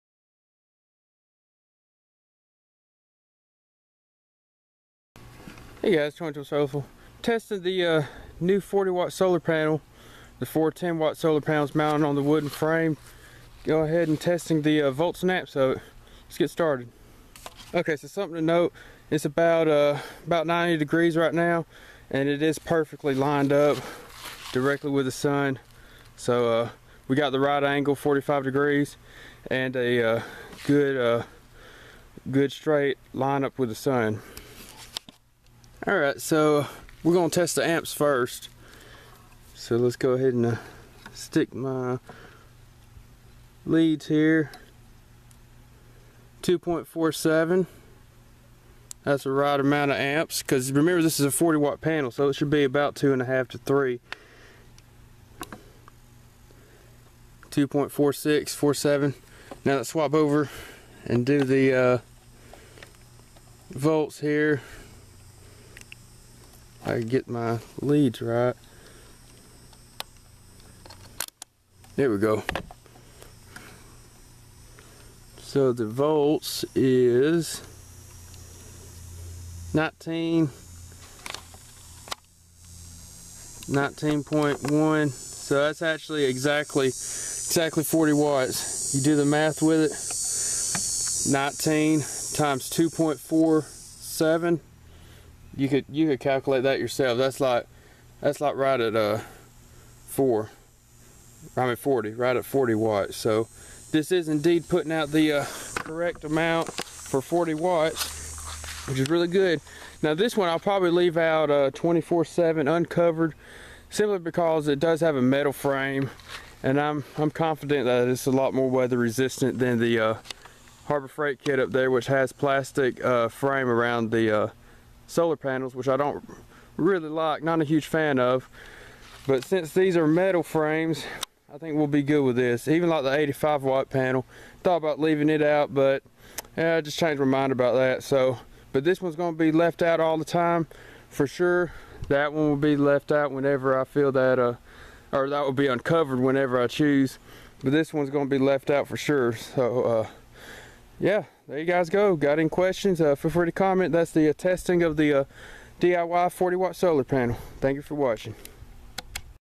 Testing The 40 Watt Solar Panel Volts And Amps Part-3
Part-3 of the DIY solar panel build. Testing the volts and amps of the 40 watt solar panel.